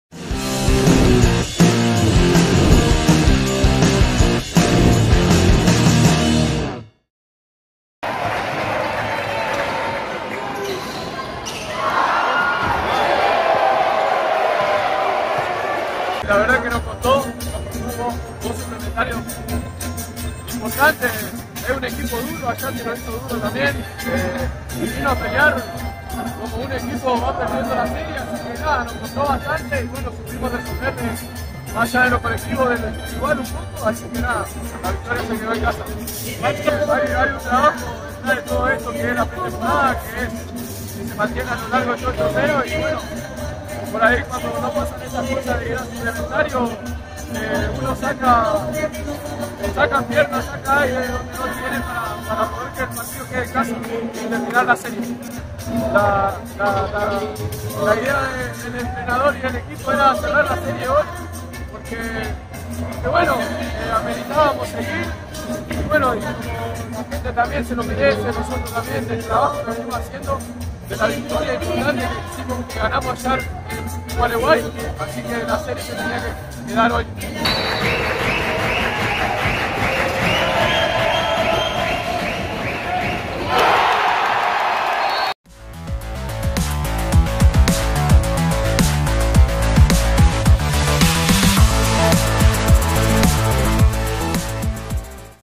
Núñez: "Nos costó bastante pero lo supimos responder"
Lo analizó Matías Núñez en referencia a la victoria en doble tiempo suplementario que significó la clasificación a semifinales.

En Posadas, Tokio derrotó a Centro Bancario de Gualeguay por 94-89, en el segundo juego de los cuartos de finales de la Interconferencia Litoral/NEA de la Liga Federal de Básquetbol 2023.